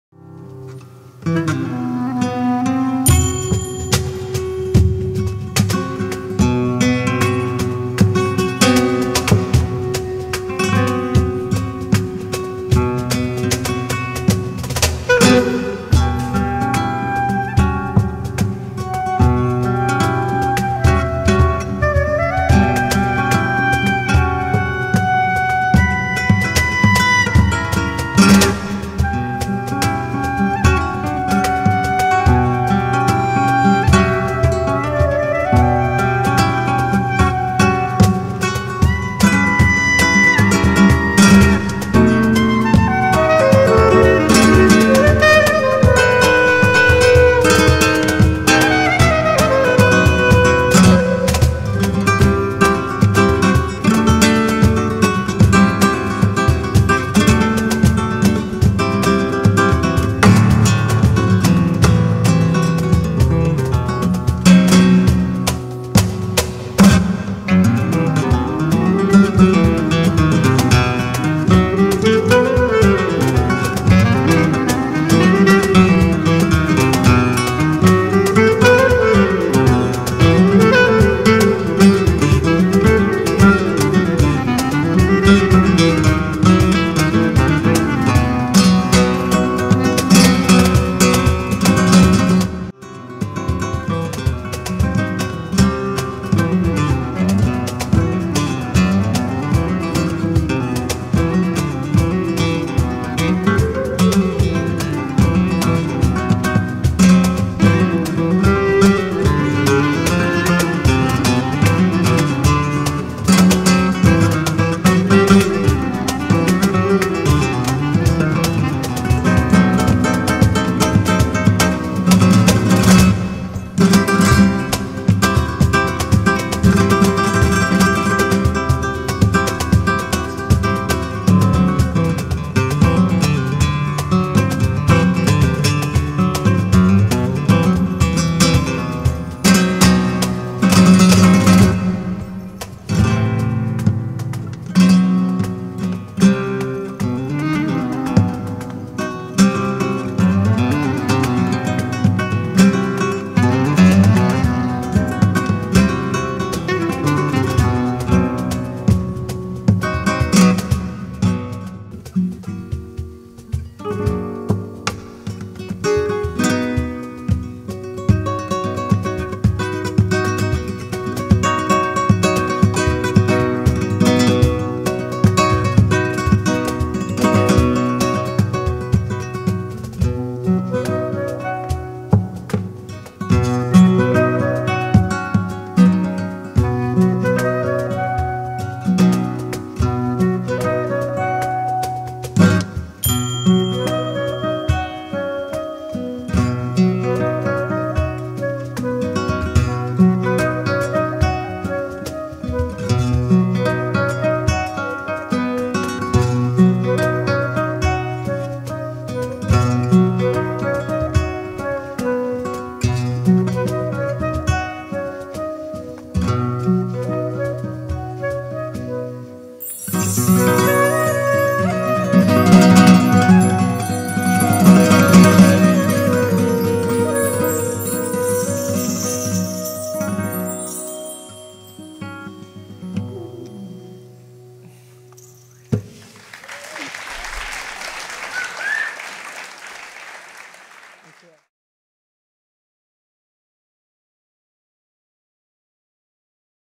Ramón Kailani Trio - Flamenco, Oriental Music, Live Bulerias
For Skype guitar lessons: info@ramonkailani, Skype: ramonkailani (Amsterdam)

Flamenco/Arabic music played live by the Ramón Kailani Trio

Guitar & Composition: Ramón Kailani
Percussion: Jacobus Thiele
Clarinet: Oscar Antoli